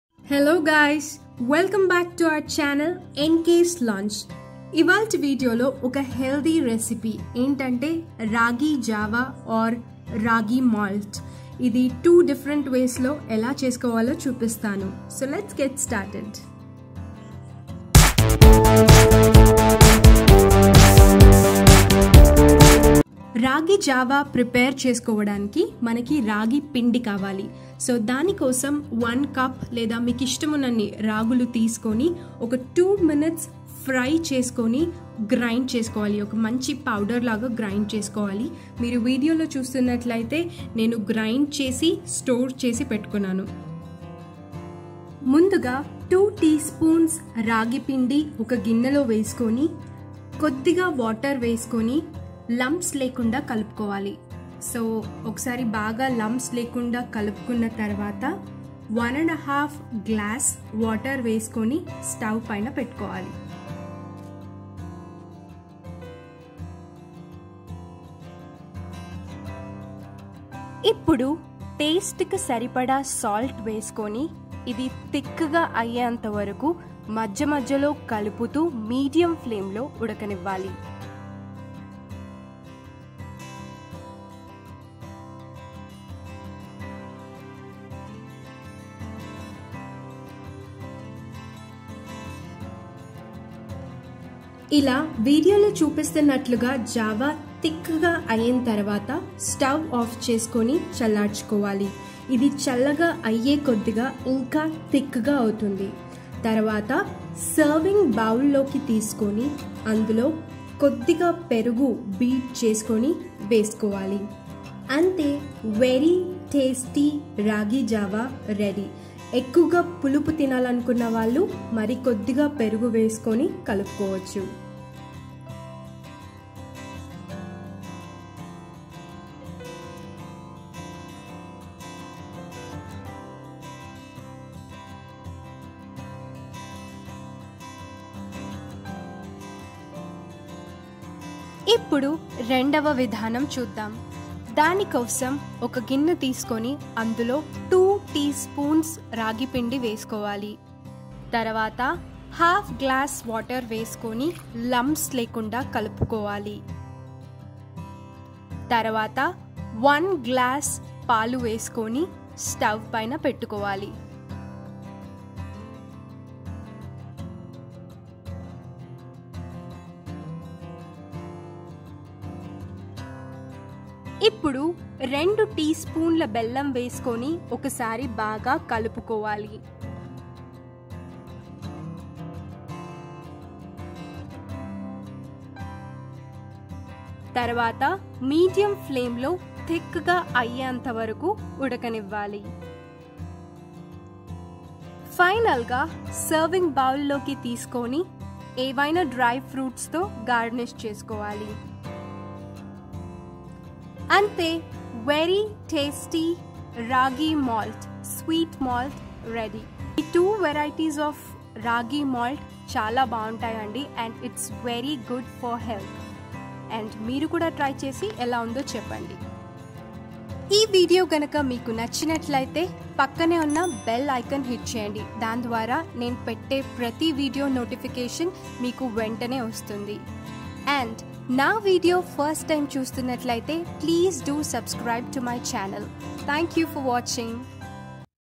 2 ways of Ragi Java| Finger Millet Malt|రాగి జావ తయారు చేయడం ఎలా?Healthy Recipes by KranthiNanda!!!
In this Video iam going to show you how to prepare Healthy Ragi Java in different ways!!!

Ingredients

1. Finger Millet Powder/  Ragi Pindi - 4 Tspns
2. Water - 2 Glasses
3. Milk - 1 Glass
4. Salt - As per Taste
5. Jaggery - 2 Tspns
6. Dry fruits - for garnish

"Copyright @ [NK's Lounge & 2020]. Any illegal reproduction of this content in any form will result in immediate action against the person concerned."